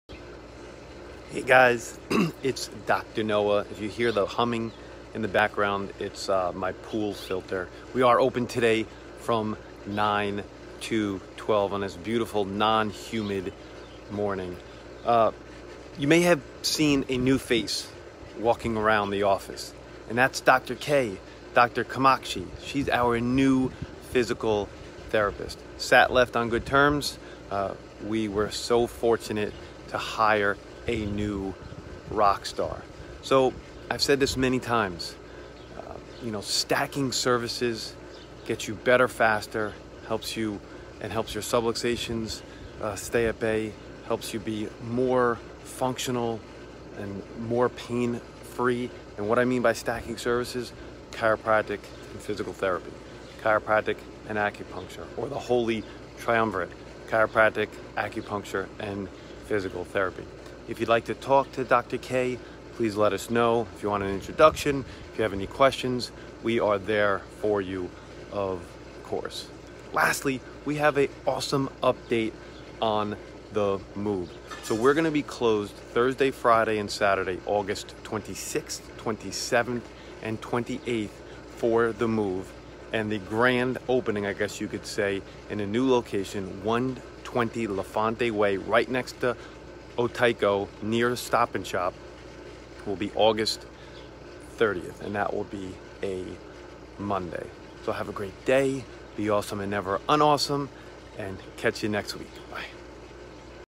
You may have seen a new face in the office.
You May Have Seen a New Face Walking Around

Welcome to my Saturday Morning FB and Instagram Live Carside Chats

We are OPEN today from 9-12

So who is the new face? The new face is our brandy new rockstar PT – Dr. Kamakshi.  Sat left us on good terms so onward and upward.  The early feedback and praise for Dr. K has been nothing but incredible.

As I have said several time stacking services such as Chiropractic and PT, or Chiropractic and Acu, or the holy triumvirate Chiro, PT, and Acu dramatically speeds recovery, rehab, and overall health and wellness.

If you have any questions regarding PT please don’t hesitate to ask us or to meet and have a quick chat with Dr. K.

Just a quick update on the move – it is progressing nicely and looks like it will be Thursday August 26th through Saturday August 28th. So the official 1st day in the new office space will be Monday August 30th. We will keep you updated!!

Dr. N